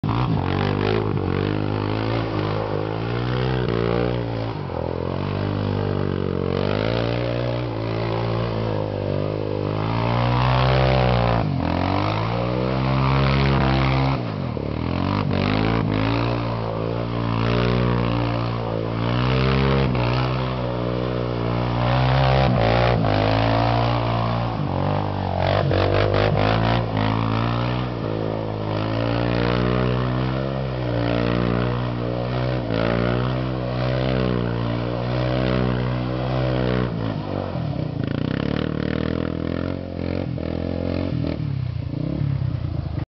Ice Donuts on the Wolvy
Rippin Donuts at the quarry in Beacon Falls